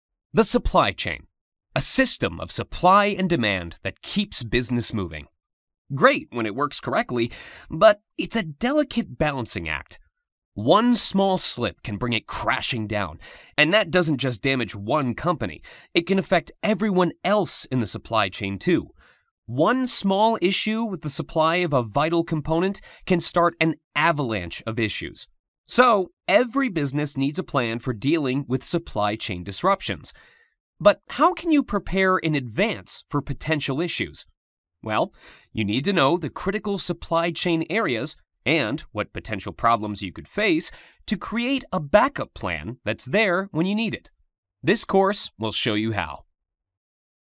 Dealing with Supply Chain Disruptions - Course Trailer - TalentLibrary™
Train your managers and leaders on dealing with supply chain interactions with this online course.

Create your free account, get access in a few clicks, and start training right away

What's covered in this course:
*Recognizing the areas of your supply chain that are critical for keeping production moving
*Identifying problems that could lead to supply disruption at each point of your supply chain
*Creating a backup plan for your supply chain that will keep critical production moving in the event of a disruption

Why your teams need this course:
Businesses need to keep moving at all times. Having a good backup plan for your supply chain will help. For managers, leaders, and those involved in the supply chain, this course will explore potential supply chain problem areas. It’ll also help you identify the places in your supply chain that would cause the biggest issues, should disruption occur, and help you plan to mitigate these.